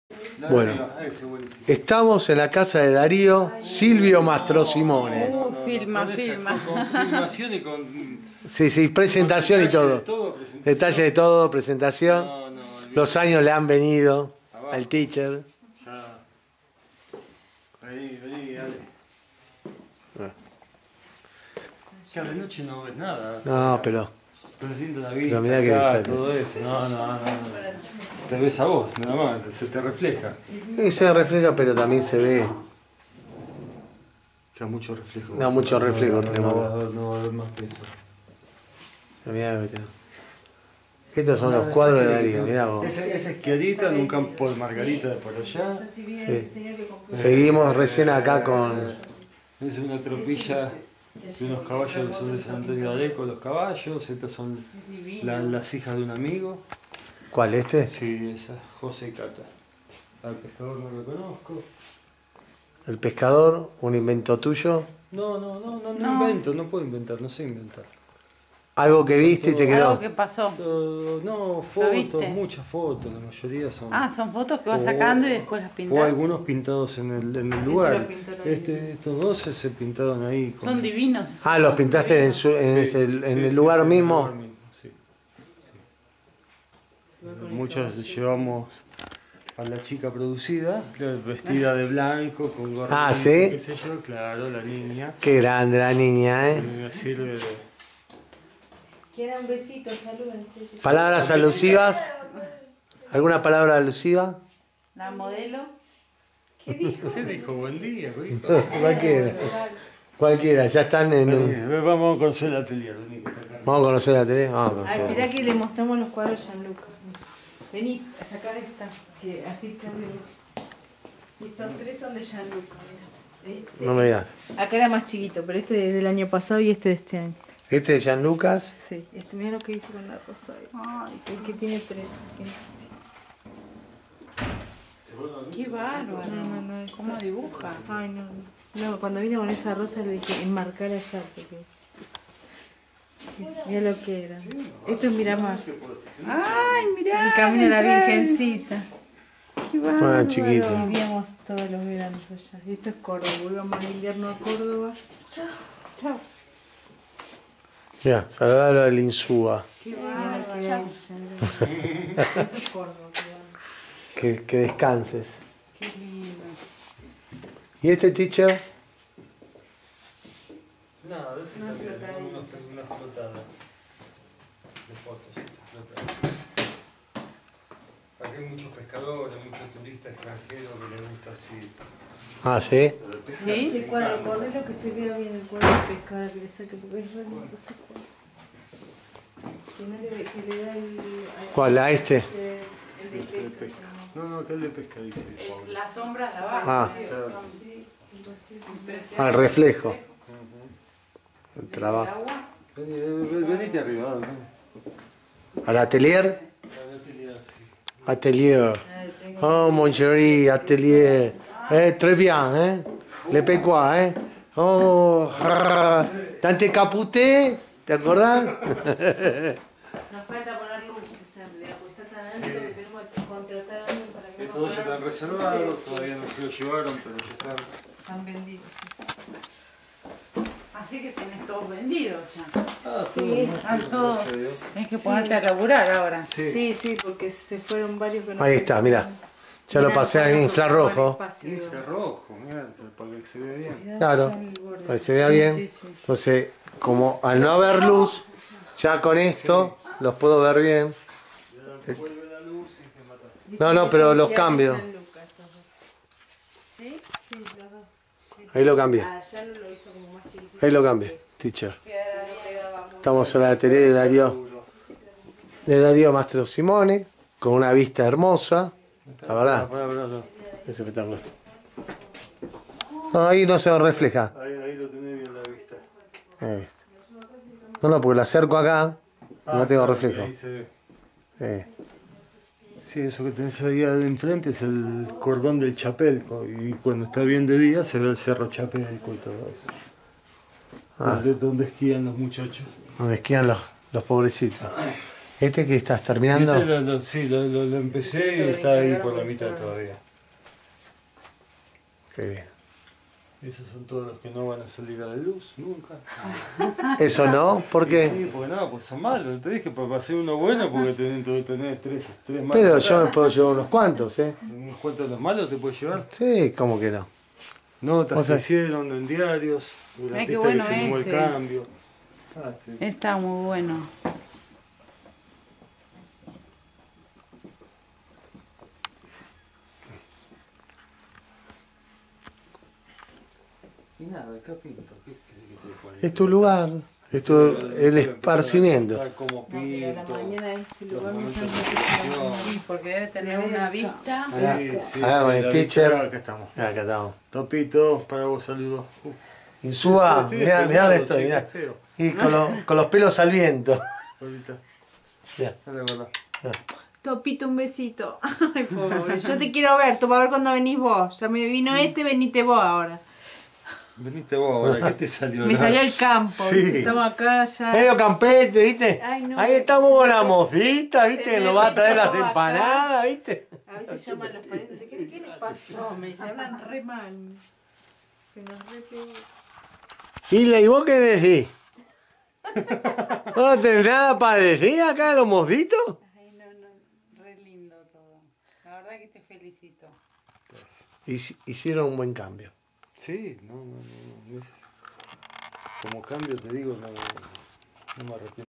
Lo Insolito!!!
l contador que dejo los numeros para dedicarce a la pintura y dejar todo e irce a San Martin de los Andes ahor es un pintor renombrado Dario Mastrosimone digno de conocer y admirar sus pinturas.